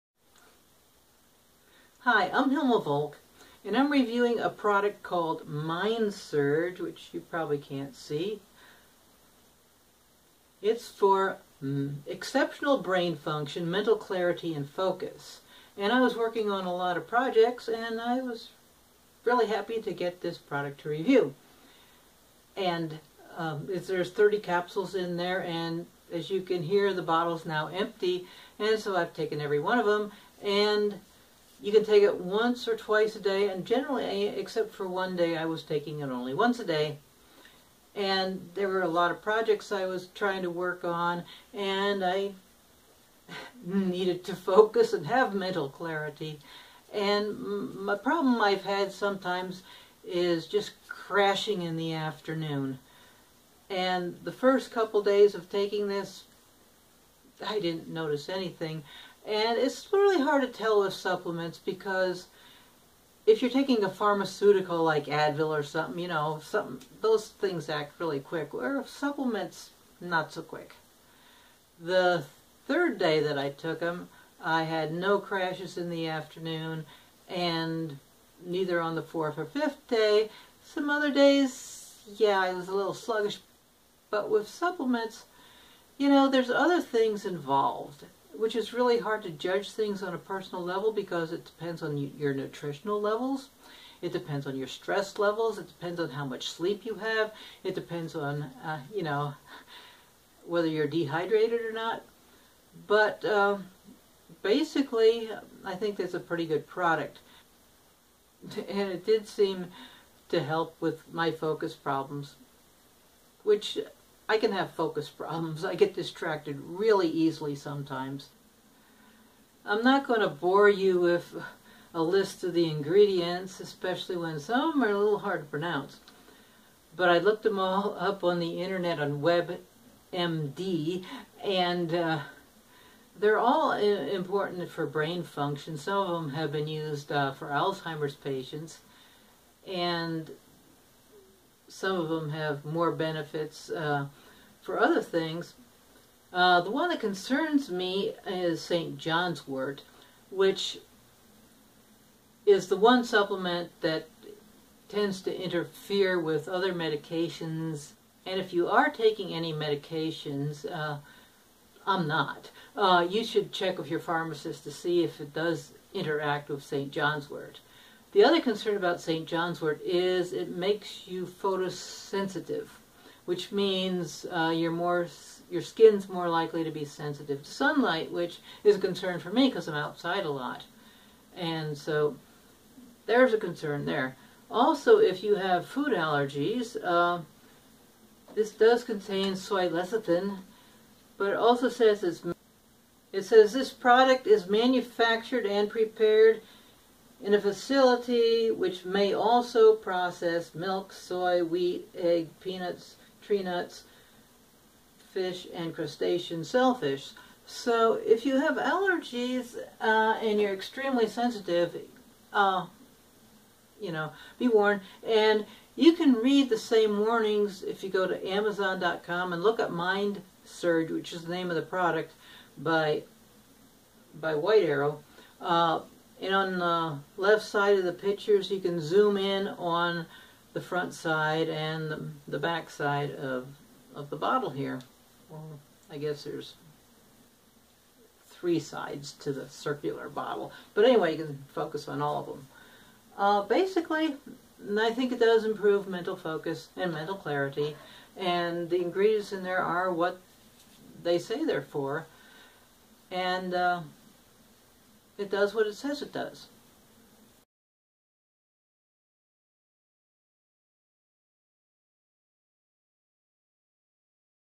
Mind Surge Review - Brain Supplement
Hilma Volk of reviews Mind Surge, a supplement by White Arrow.  You can find it on Amazon.com . It's purpose is to support brain function, mental clarity, and focus.